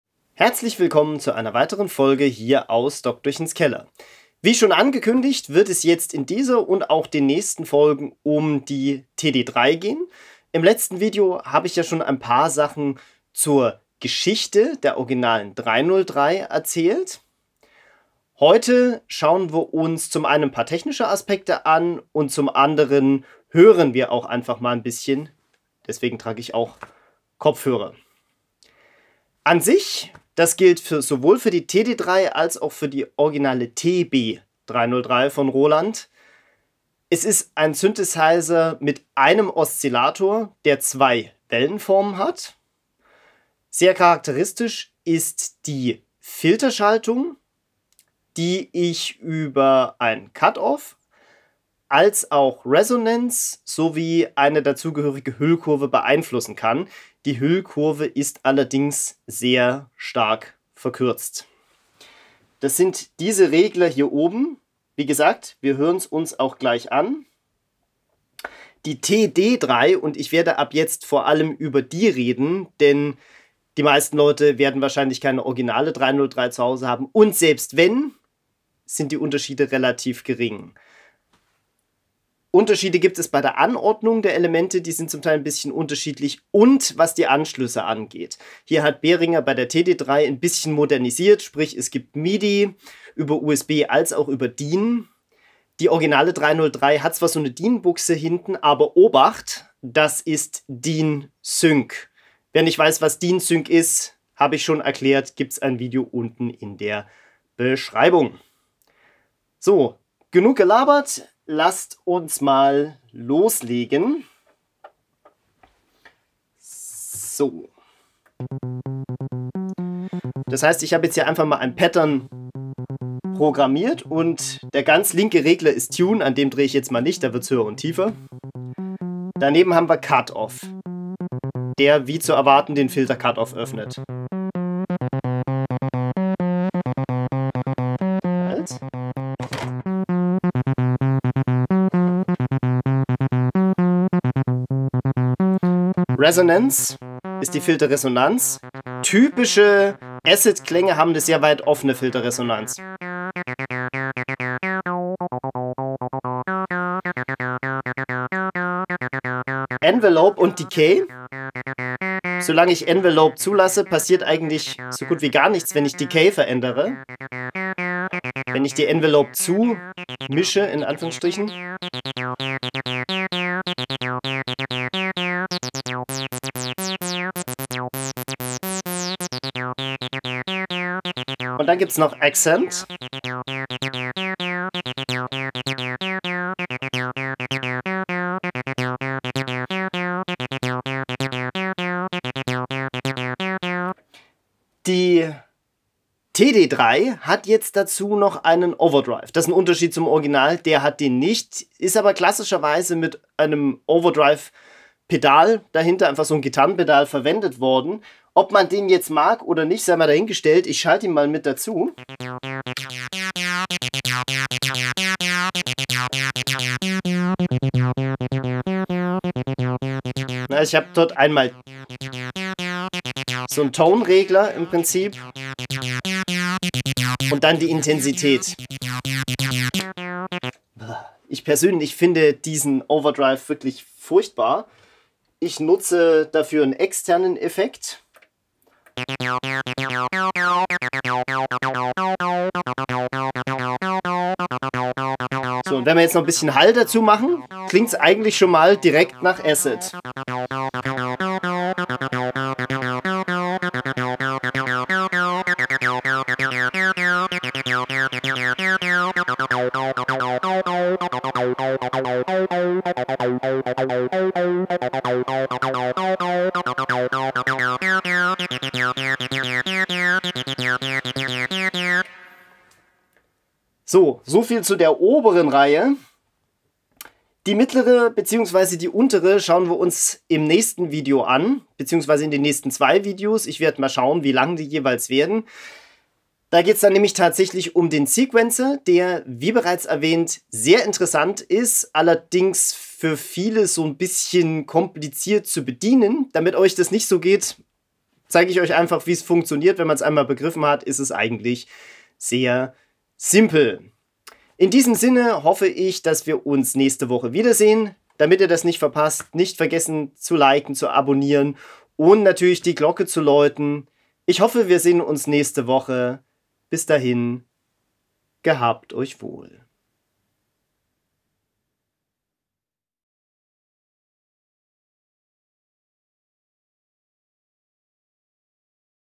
Die Acid-Regler…es wird gefiltert | TD3 / TB303
In diesem Video schauen und hören wir uns ein paar Bedienelemente der TD3 und deren Funktion an.

Schreibt mir gern einen Kommentar.